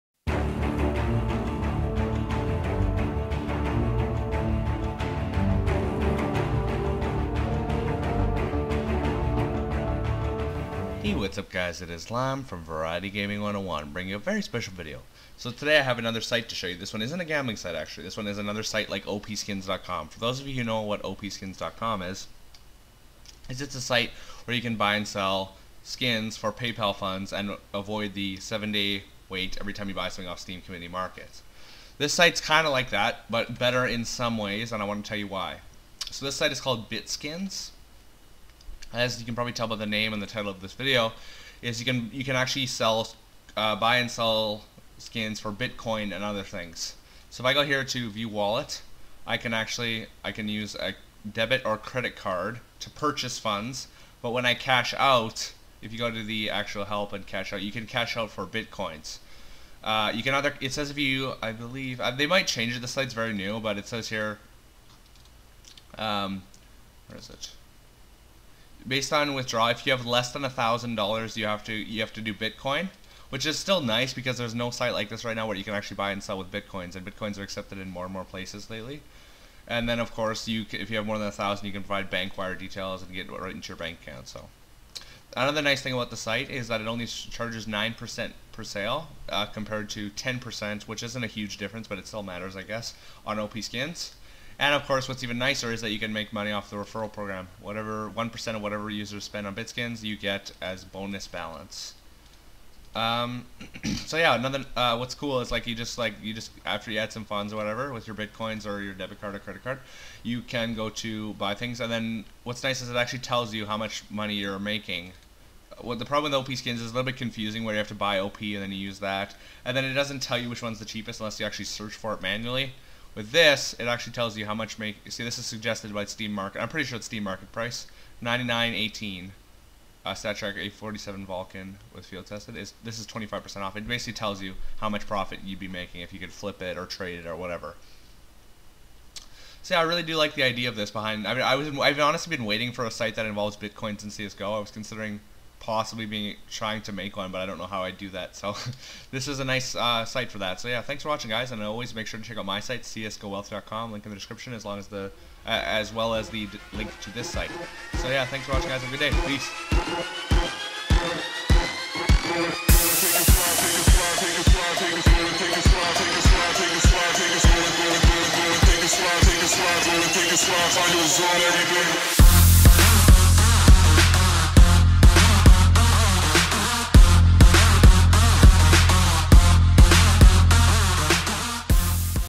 How to Buy/Sell CSGO Skins for BITCOINS! (And Credit Cards)
The first CSGO site that brings bitcoins into the fray!